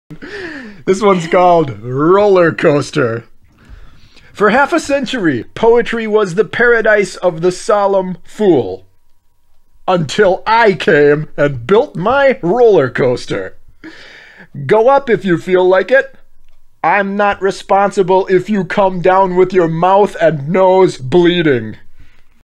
Nicanor Parra - ROLLER COASTER [in English translation]
A casual reading of Nicanor Parra's LA MONTANA RUSA, translated by Miller Williams.